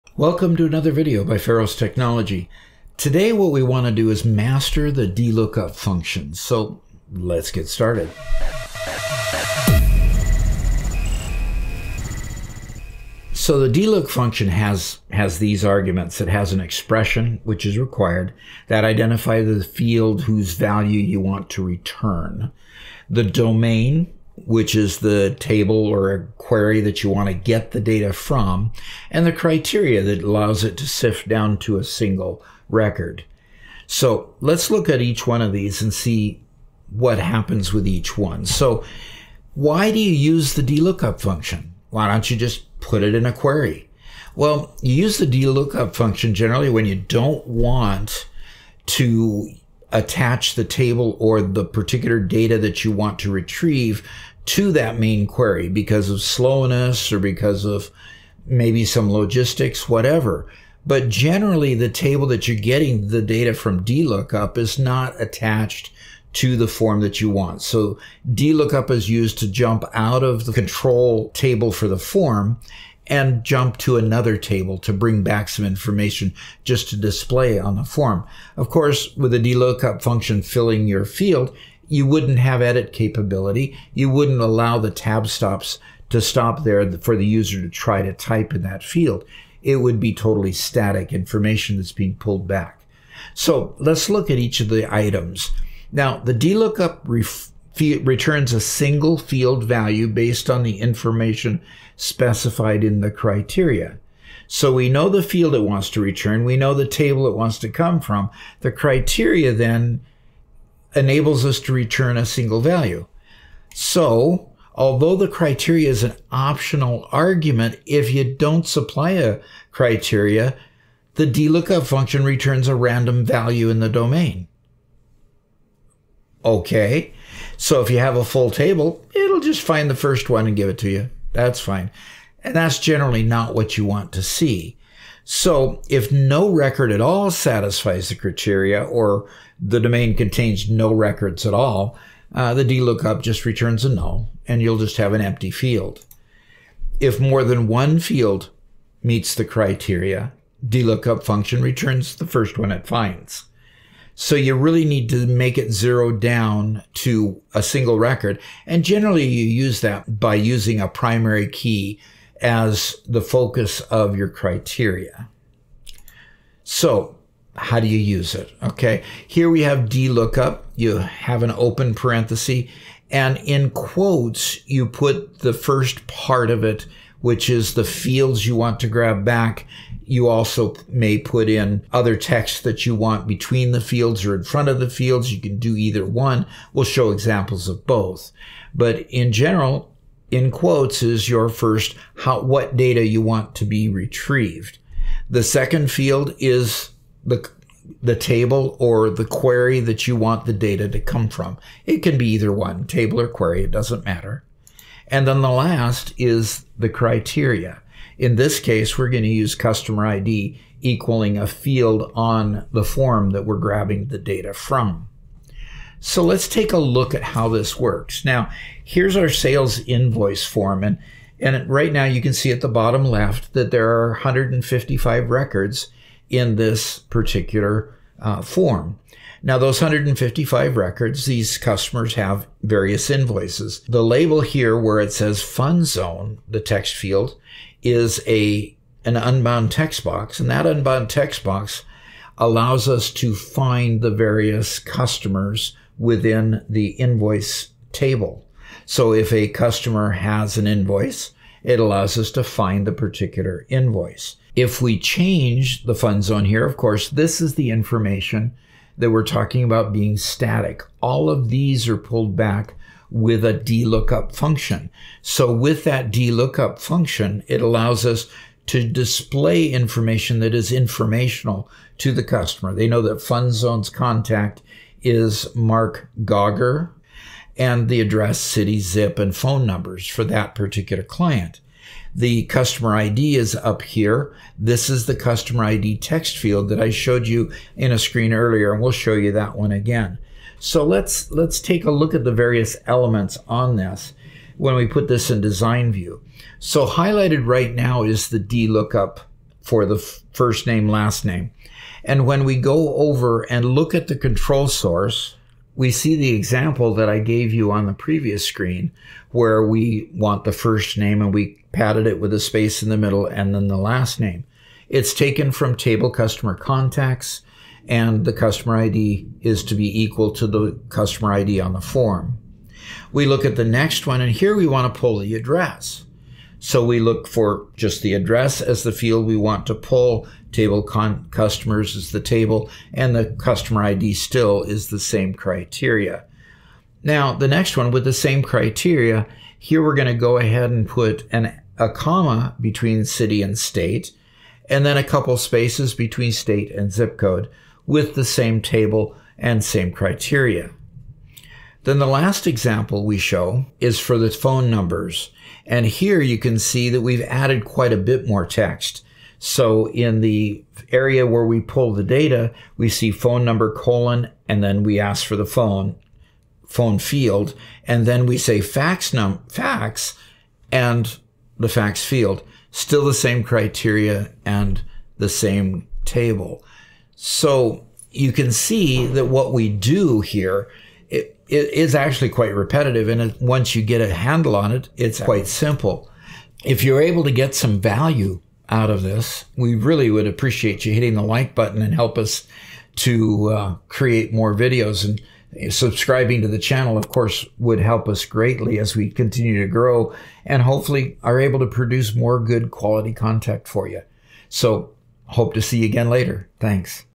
Mastering the DLookup Function for Form-Level Data
This video will help you to master the DLookup function, especially when you don't want to connect a particular table or data to your main query due to potential performance issues or logistical concerns. Dive deep into the details of the DLookup function: from understanding its arguments like expression, domain, and criteria, to its application in real-world examples. Discover how DLookup can be used to retrieve specific information from another table without linking it directly, the nuances of the criteria parameter, and practical demonstrations on a sales invoice form. With this video, gain clarity on how to effectively use the DLookup function in your database projects.